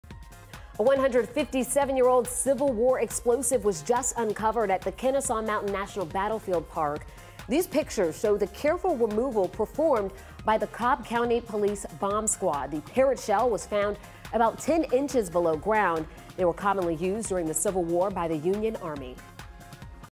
157-year-old Civil War explosive found at metro Atlanta battlefield
The department said bomb technicians carefully dug it out, examined it, and then moved it to a bunker for storage, where it will later be counter-charged.